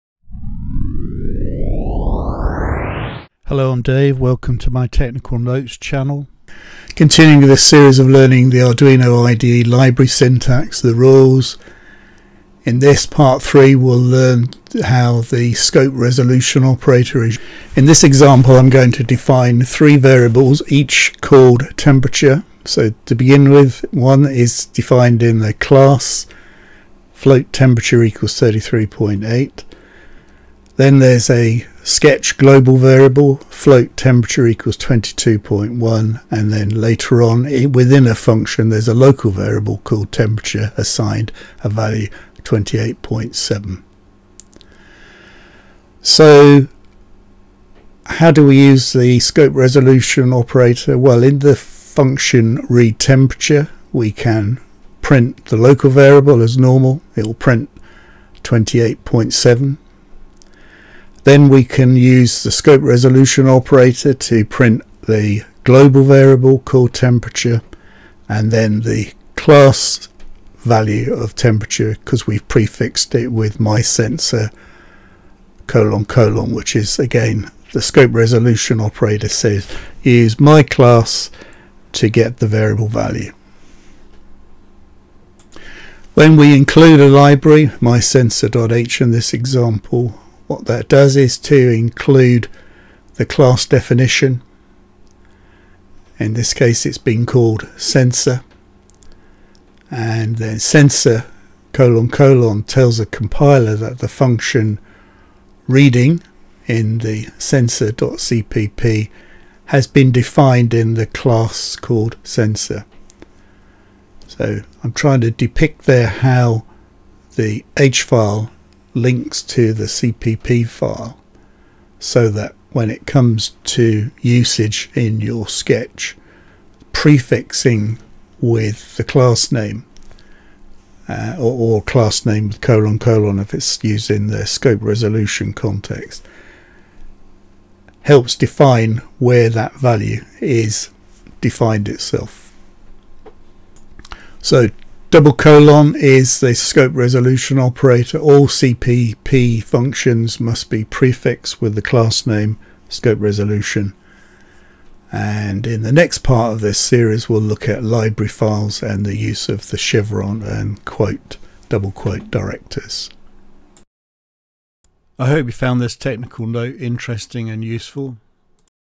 Tech Note 119 - Scope Resolution Operators used in Libraries (Part 3 of 4)
The use of Scope Resolution Operators is an important part of creating an Arduino IDE library, here I go through some examples.